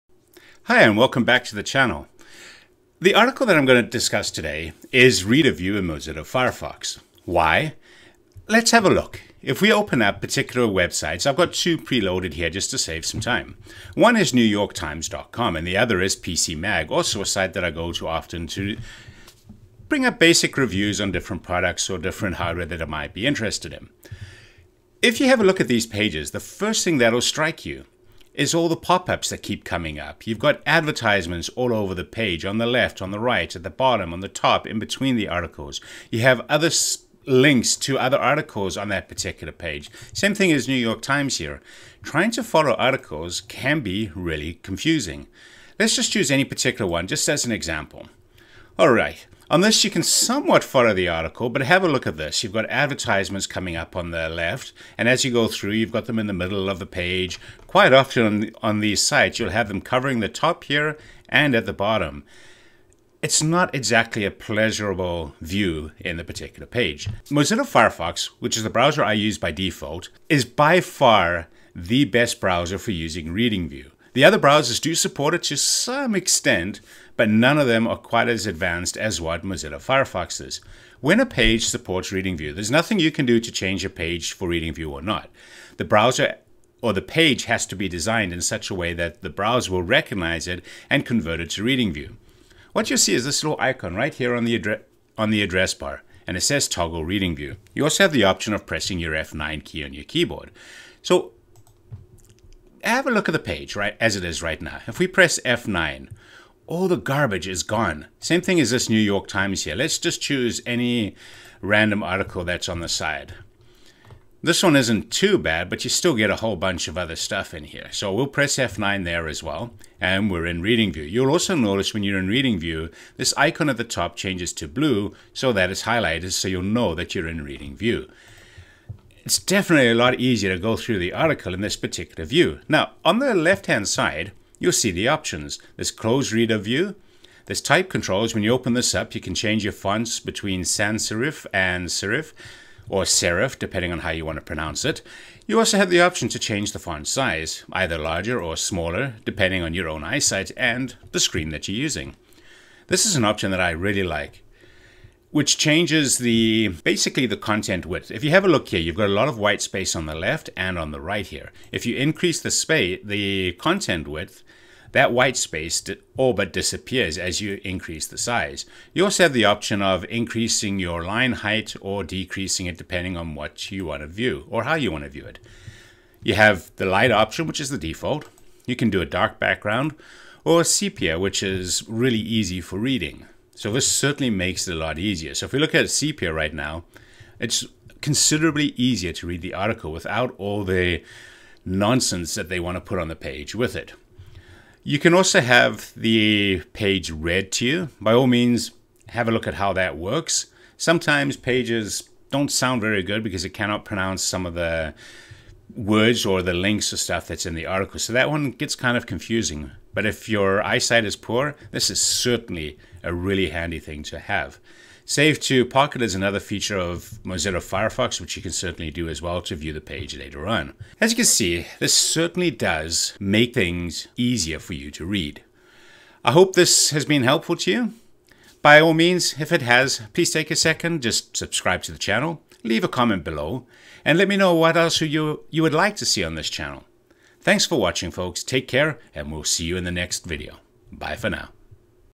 Reader View in Mozilla Firefox
There are far too many web pages that bombard you with banners, ads, videos and other distracting, unrelated stuff.  Sometimes all you want is to simply read the article - Enter in "Reading View".  This simplifies the page and in many cases offers other options too like reading the article for you.